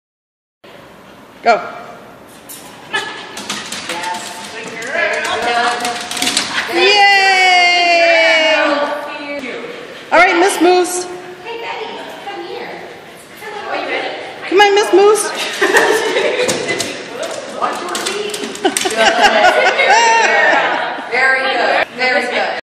First time walking the board, no trouble at all
First day of agility training.  No problem for the sure footed keeshond.